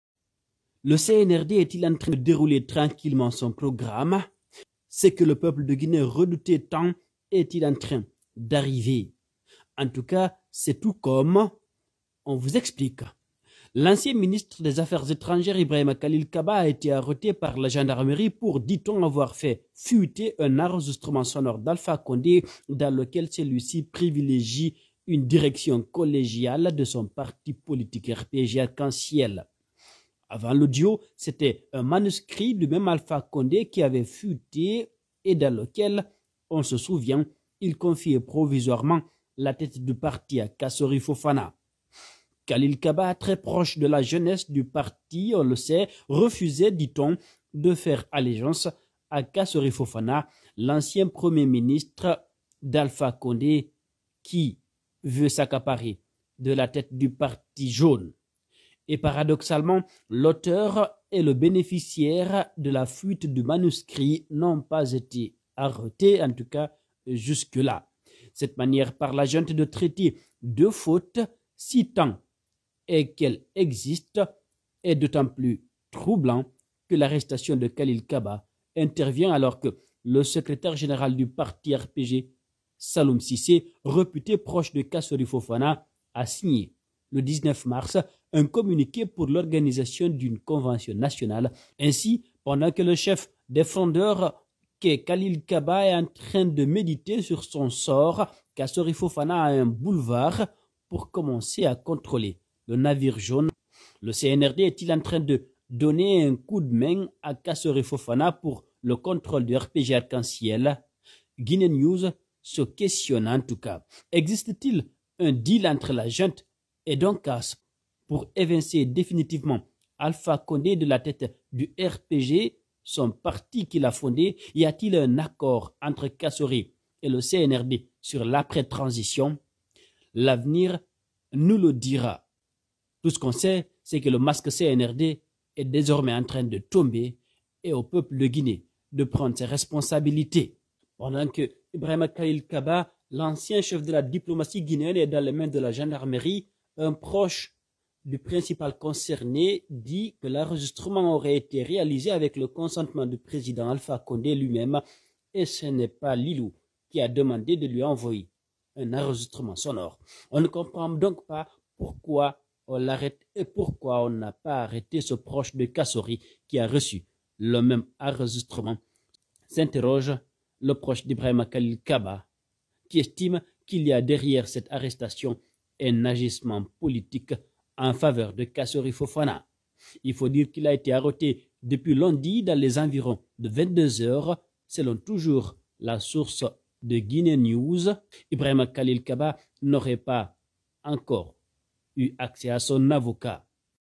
Guinée : voici le plan du CNRD pour soutenir son candidat. On vous explique tout !
Audio Alpha Condé : Le plan du CNRD pour soutenir son candidat. On vous explique tout !